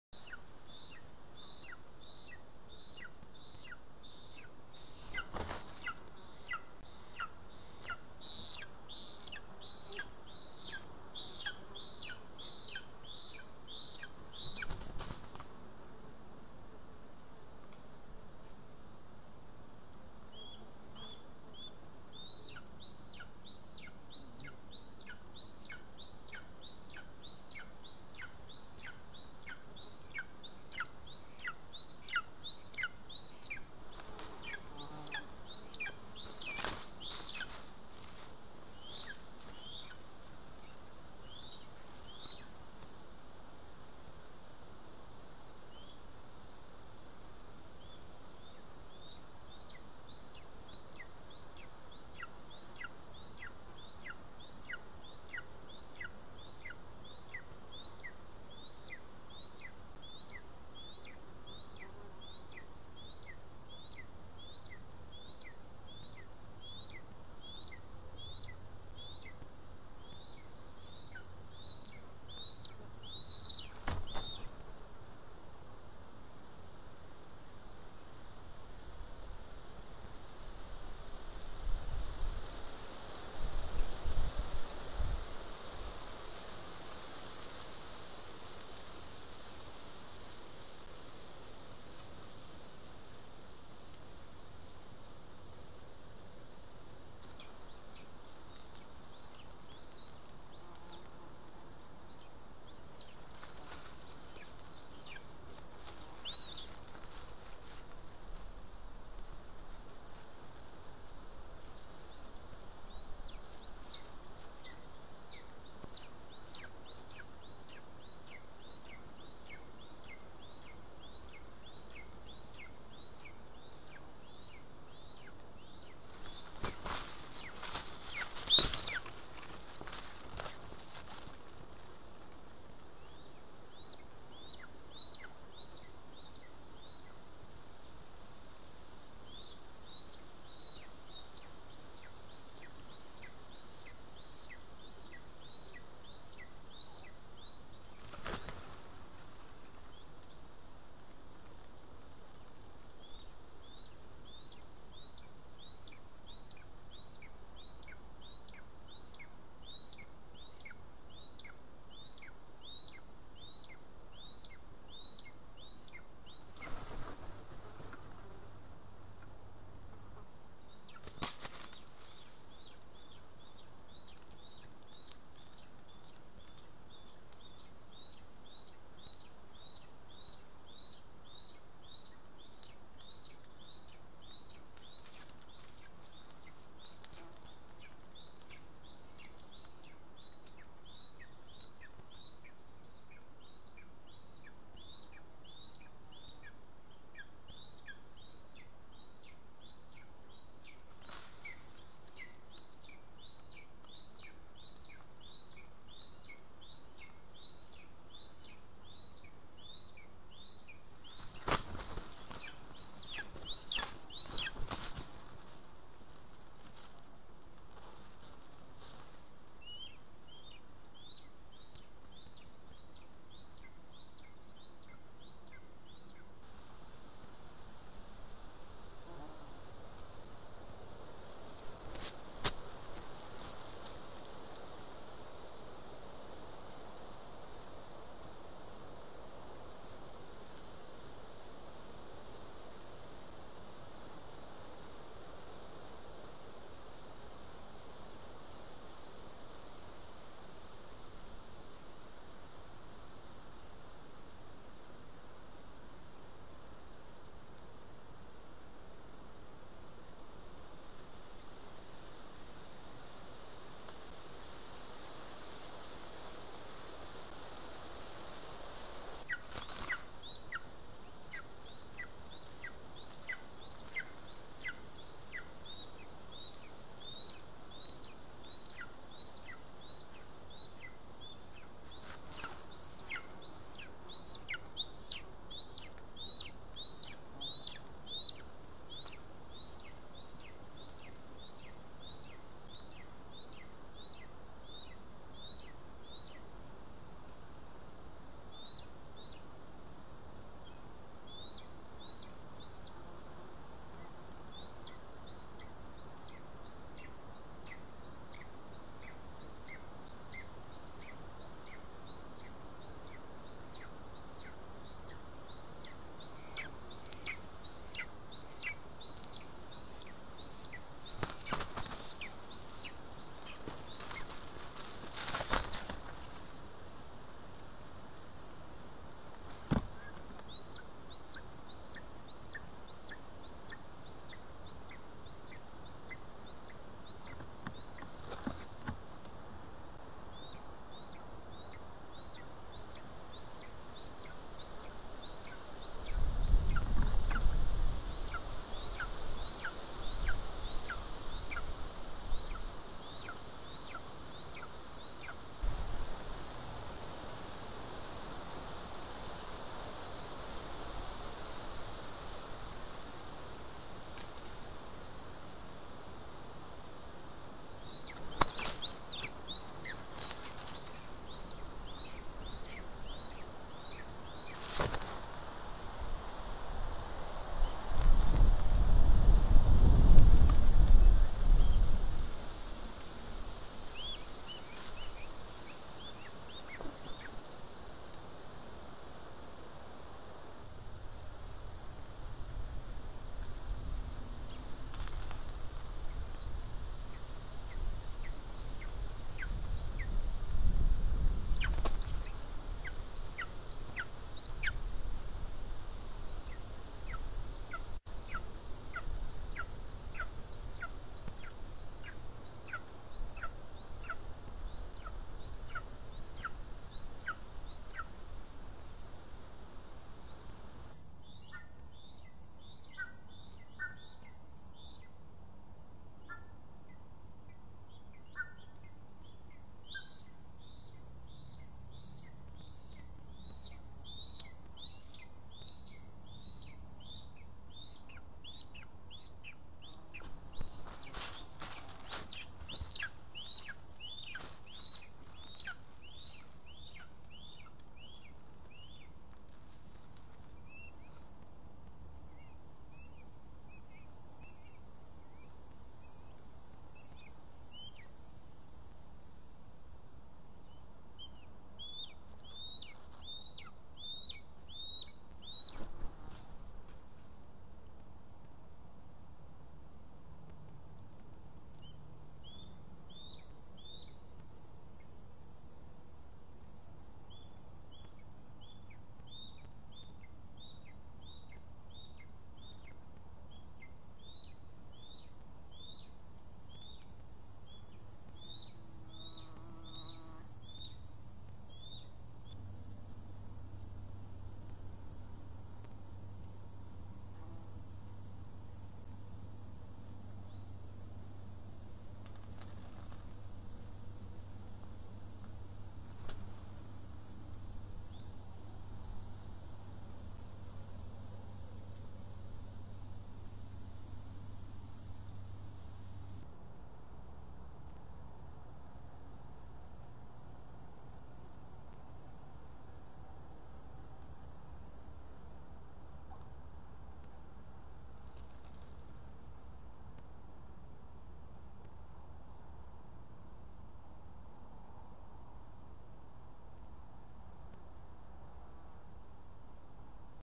2020 08 02 Estonia Lesser Spotted Eagle ~ Vole Party For Raju
Some of the 13! voles today